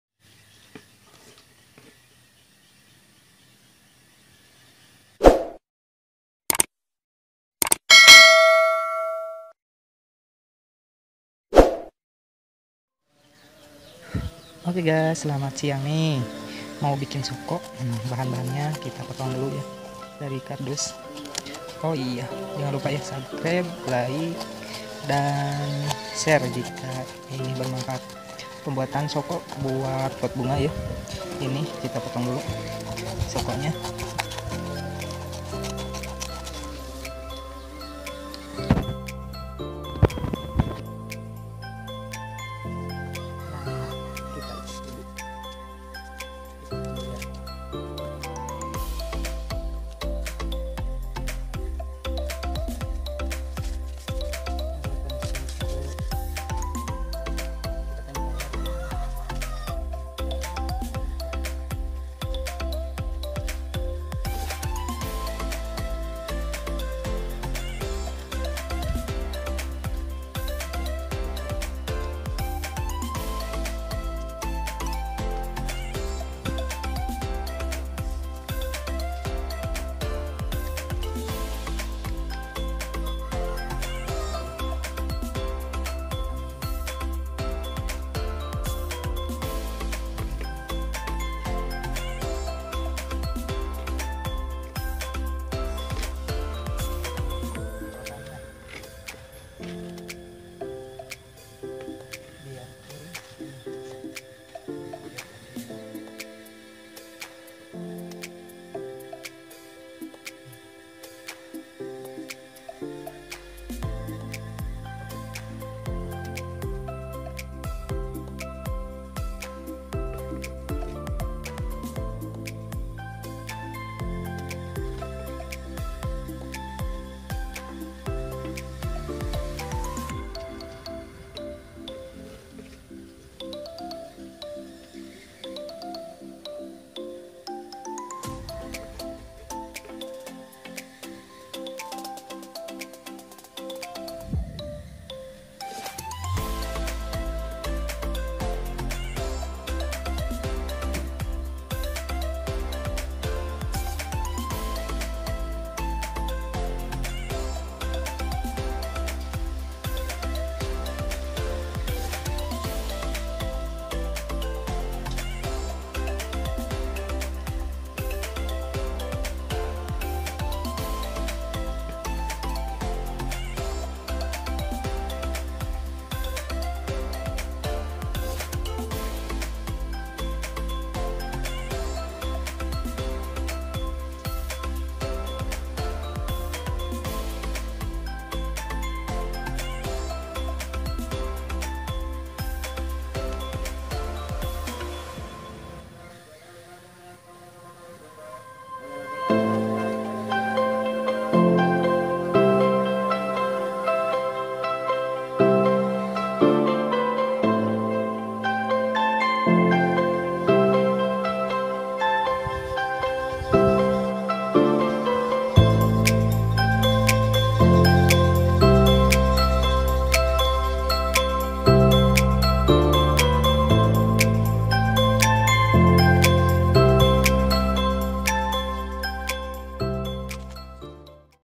cara pembuatan soko pot bunga
Hallo kawan selamat datang di cahannel saya.
Naah cara dan pembuatan kita siapkan bahan bahan yaa dulu.kita potong kardus yang sudah di sediakan.
Lalu kita ikat dengan tali dengan persegi empat.
Lalu siapkan bahan pengecoran nya dari semen yang sudah di aduk.
Lalu masukan adukanan semen nya..
Terima kasih yang sudah menyimak.